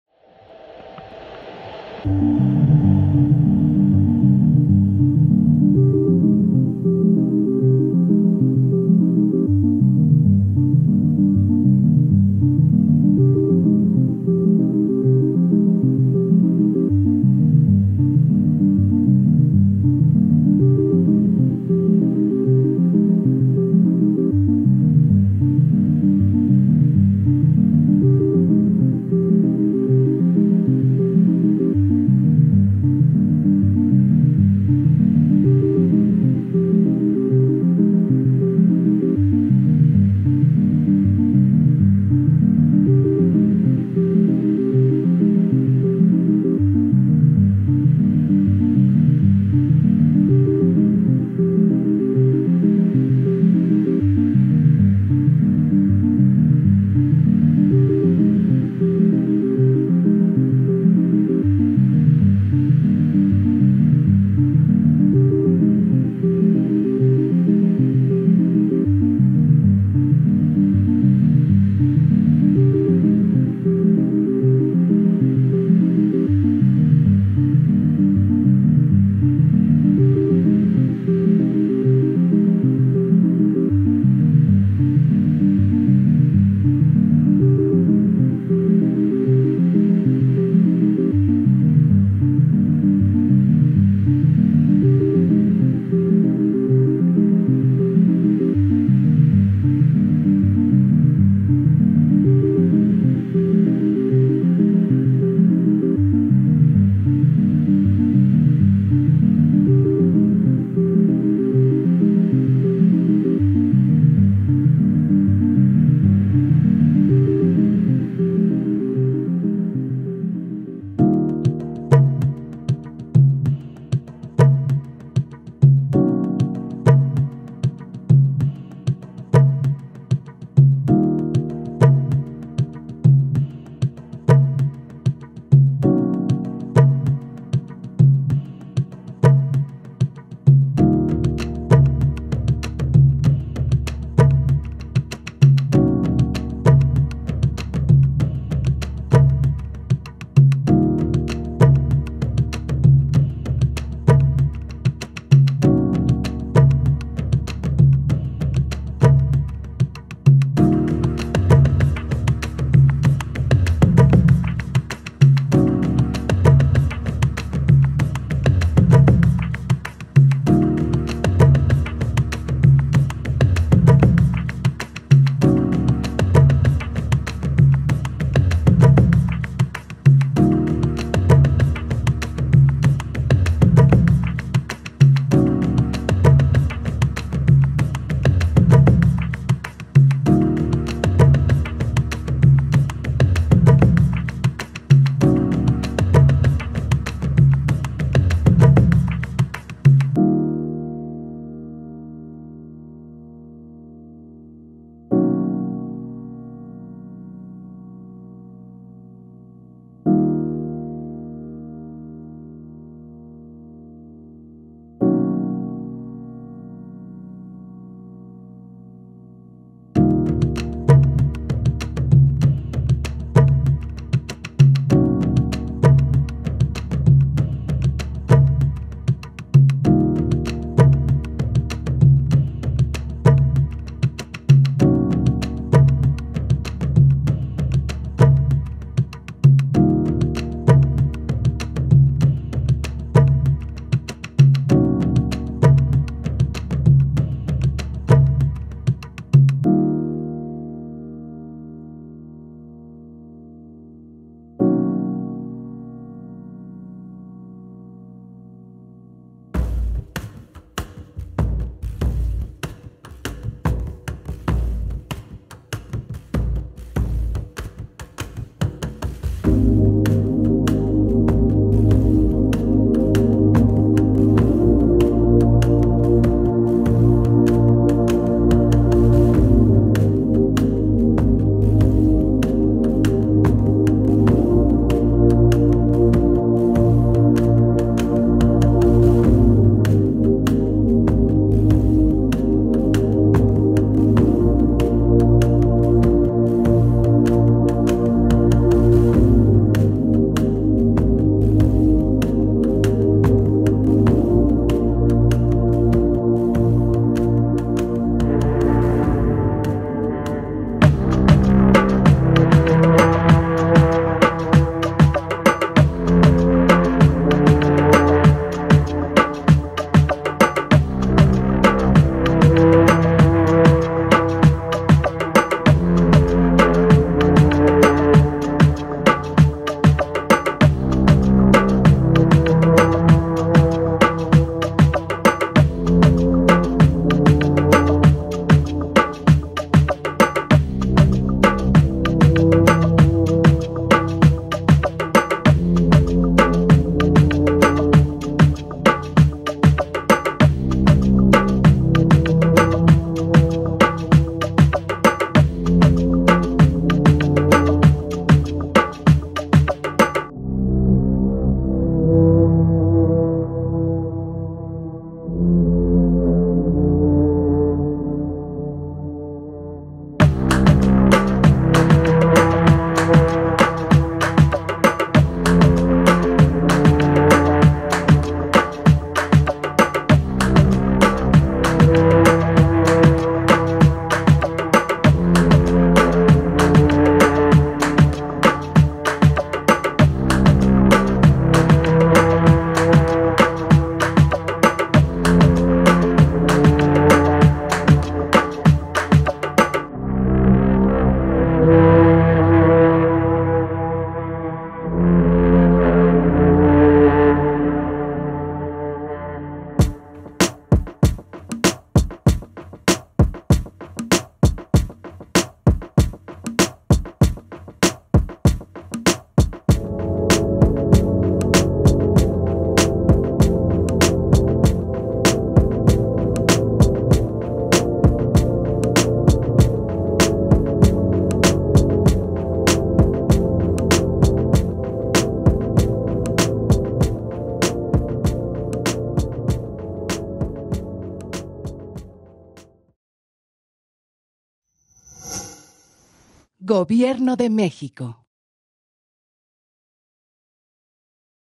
"Murales vivos. Laboratorio audiovisual" impartido en La Tallera
Como resultado de su aprendizaje en el curso de verano "Murales vivos. Laboratorio audiovisual" impartido en 2024 en La Tallera, las niñas y los niños participantes realizaron esta historia animada, con la guía de Luis Felipe Hernández Alanis, tallerista.